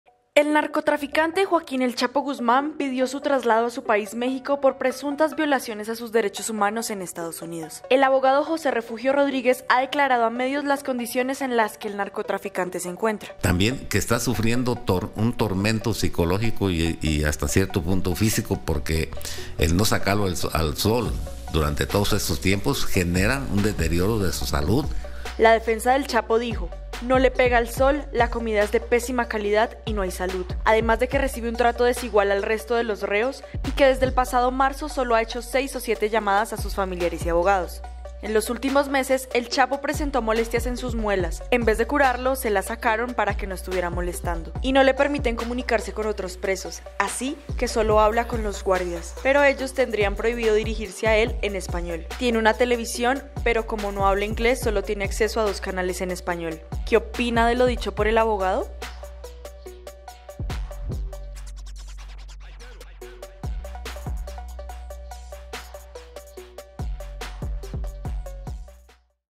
"El Chapo" Guzmán: sin sol, ni visitas, así vive el narcotraficante
Guzmán sale apenas tres veces por semana a un "corralito" de unos cinco metros cuadrados donde "no le pega el sol", dice su abogado.  Le contamos todos los detalles.

¿Qué opina de lo dicho por su abogado?